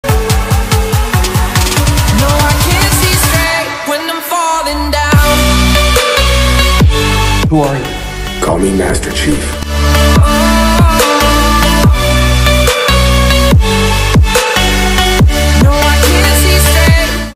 Master Chief wearable armor suit made with foam.
Screen accurate, made to measurements master chief cosplay suit.